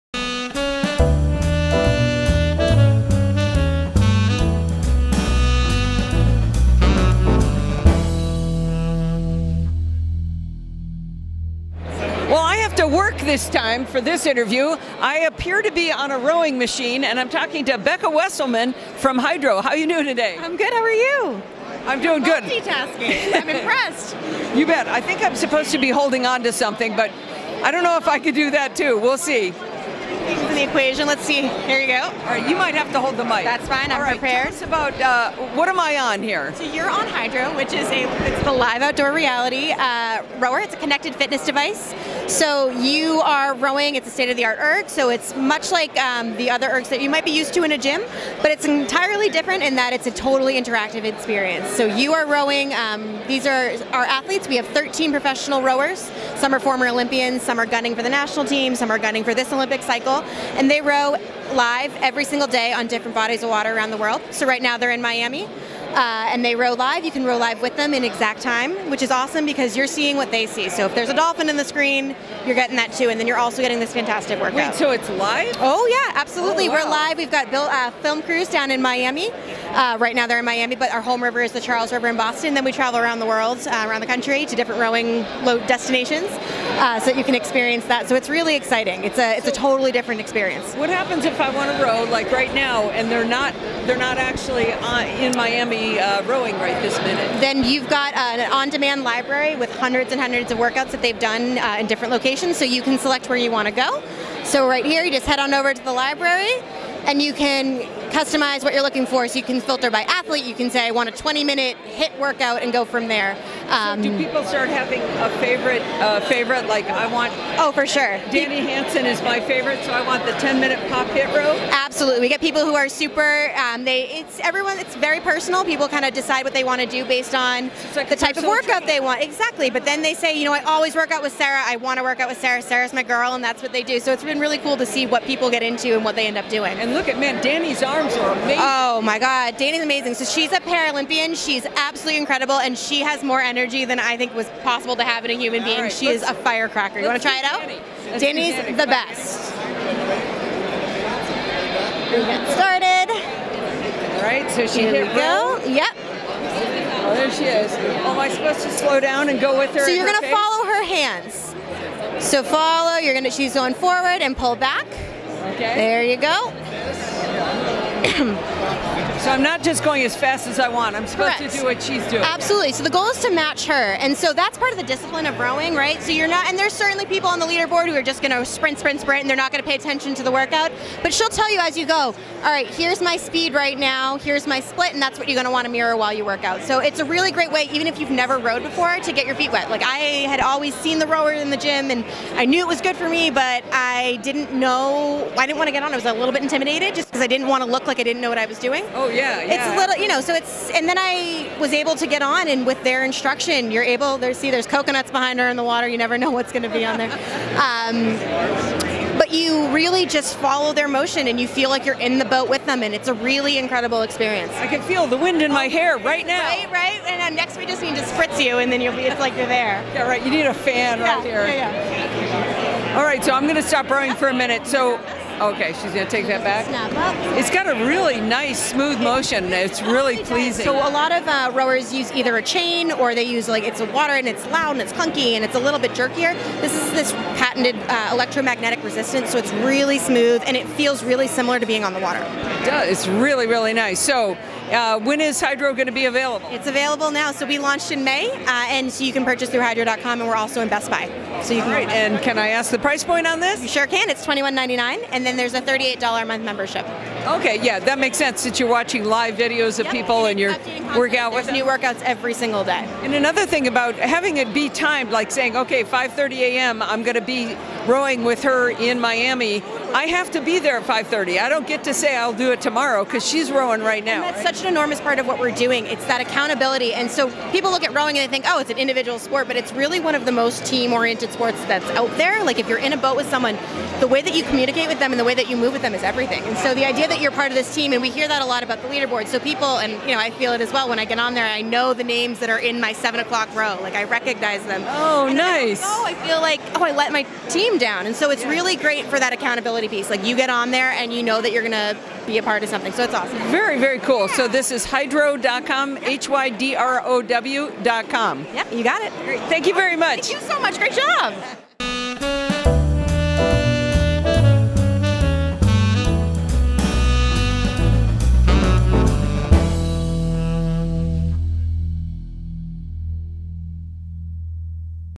CES 2020: Hydrow Live Outdoor Reality Rower
Allison Sheridan interviews Becca Mortimer from Hydrow about their Live Outdoor Reality Rower.  The Hydrow is a state-of-the-art rowing machine that includes a 22” sweat- and dust-resistant touchscreen with front-facing speakers. The display provides guided rowing workouts and an immersive experience in your home. You can even join a crew as one on the water and follow their rhythm as they lead you through live and on-demand workouts. Hydrow is ergonomically designed with an aluminum frame and an industrial grade webbed strap for smooth and quiet strokes. Its 10-roller system seat is contoured and cushioned for support and comfort. The setting is Pepcom's Digital Experience at CES at the Mirage Hotel in Las Vegas.